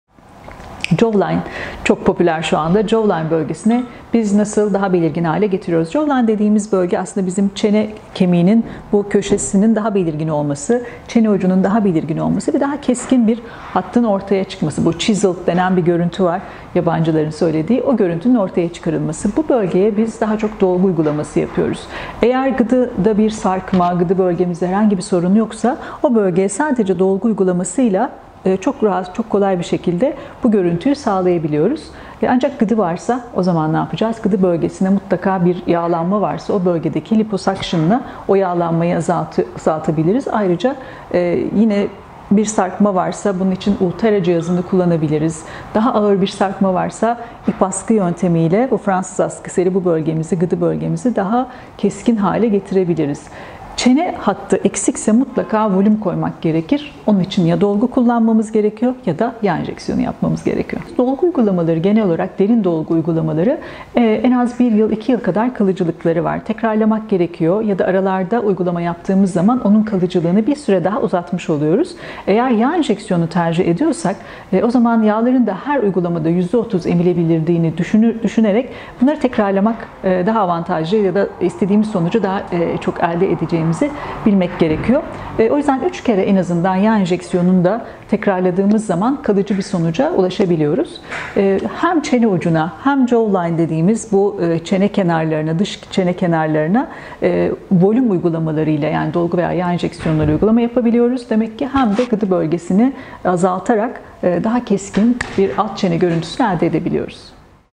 ÇENE HATTI (JAWLINE) NASIL BELİRGİNLEŞİR?! | Op. Dr. Leyla Arvas
Jawline çok popüler şuanda Jawline bölgesini biz nasıl daha belirgin hale getiriyoruz? Jawline dediğimiz bölge aslında bizim çene kemiğinin bu köşesinin daha belirgin olması, çene ucunun daha belirgin olması ve daha keskin bir hattın ortaya çıkması. Chiseled denen bir görüntü var. Yabancıların söylediği o görüntünün ortaya çıkarılması bu bölgeye biz daha çok dolgu uygulaması yapıyoruz. Eğer gıdıda bir sarkma, gıdı bölgenize herhangi bir sorun yoksa o bölge sadece dolgu uygulamasıyla çok rahat, çok kolay bir şekilde bu görüntü sağlayabiliyoruz. Ancak gıdı varsa ozaman ne yapacağız? Gıdı bölgesine mutlaka bir yağlanma varsa o bölgedeki liposuctionla o yağlanmayı azaltabiliriz. Ayrıca yine bir sarkma varsa bunun için Ulthera cihazını kullanabiliriz. Daha ağır bir sarkma varsa ip askı yöntemiyle bu Fransız askısıyla bu bölgemizi gıdı bölgenizi daha keskin hale getirebiliriz. Çene hattı eksikse mutlaka volüm koymak gerekir onun için ya dolgu kullanmamız gerekiyor yada yağ enjeksiyonu yapmamız gerekir. Dolgu uygulamaları genel olarak derin dolgu uygulamaları en az bir yıl iki yıl kadar kalıcılıkları var. Tekrarlamak gerekiyor ya da aralarda uygulama yaptığımız zaman onun kalıcılığını bir süre daha uzatmış oluyoruz. Eğer yağ enjeksiyonu tercih ediyorsak ve o zaman yağların da her uygulamada %30 emilebildiğini düşünerek bunları tekrarlamak, daha avantajlı ya da istediğimiz sonucu daha çok elde edeceğimizi bilmek gerekiyor. O yüzden üç kere en azından yağ enjeksiyonunu tekrarladığımız zaman kalıcı bir sonuca ulaşabiliyoruz. Hem çene ucuna hem Jawline dediğimiz bu çene kenarlarına dış çene kenarlarına volüm uygulamaları yaparak dolgu veya yağ enjeksiyonua yapabiliyoruz demekki hem de gıdı bölgesini azaltarak daha keskin bir alt çene görüntüsü elde ede biliyoruz.